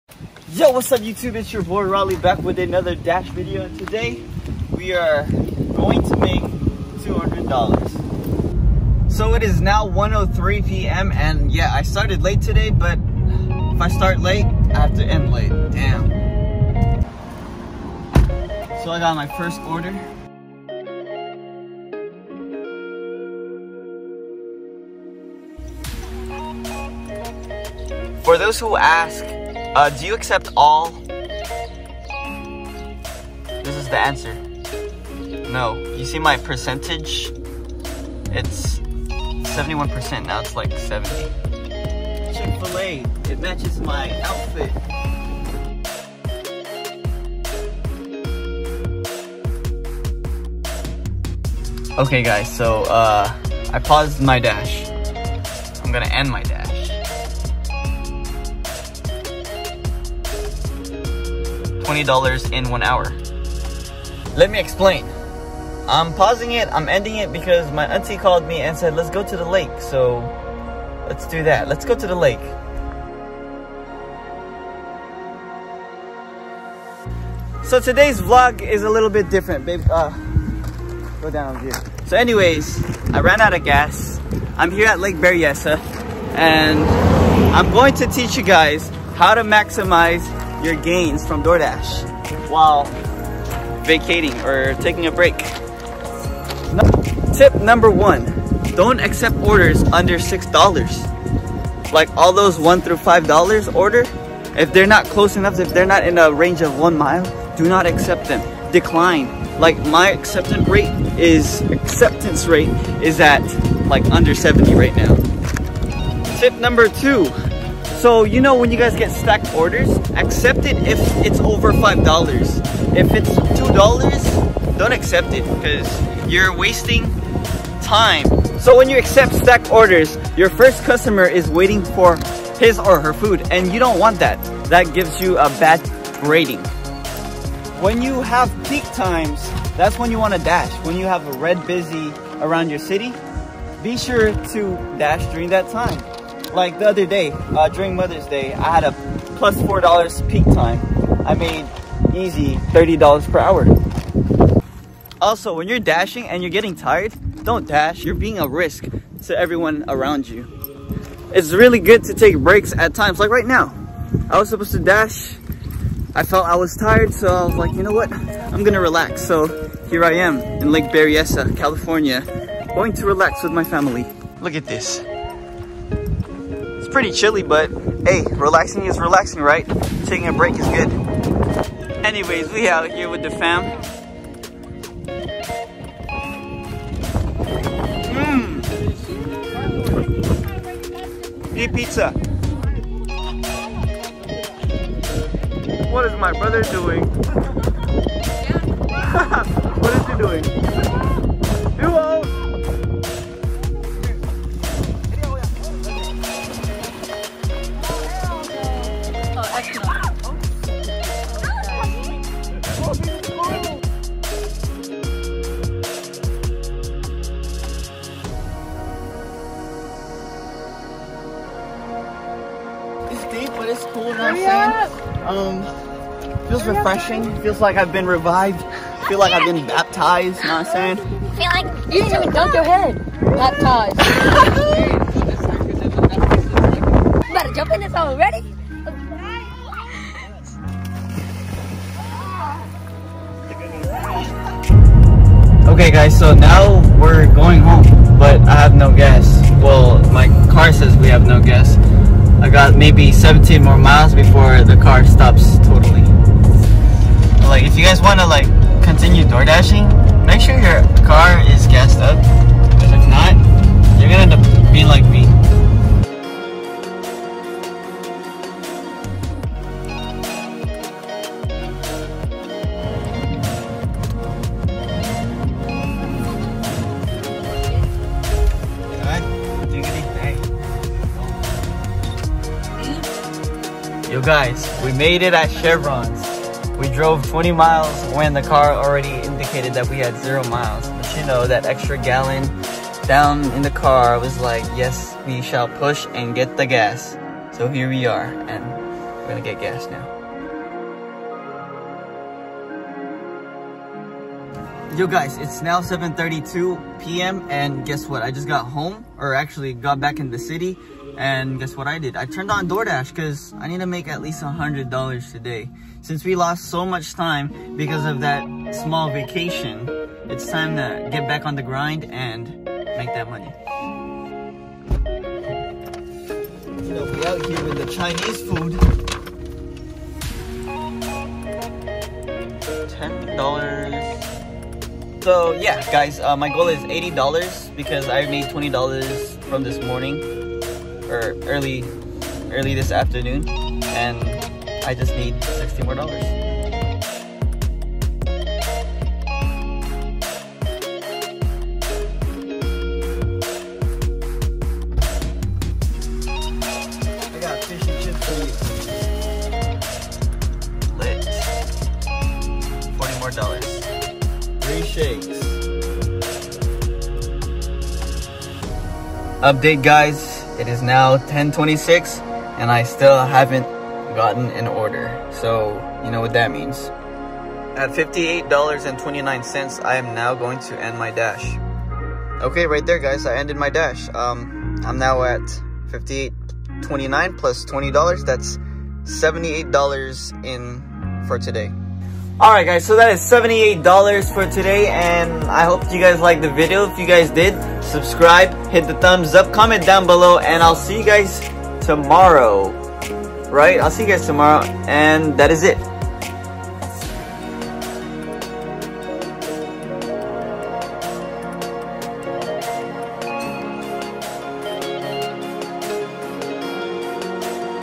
Doordash | 20$ Per Hour, Tips, and Taking Breaks EP10
I made 80$ today in 4 hours of doordash. I also took a break at Lake Somethn ahhahaaha. Swam for a few mins and I guess vlogged a bit. Hope you enjoy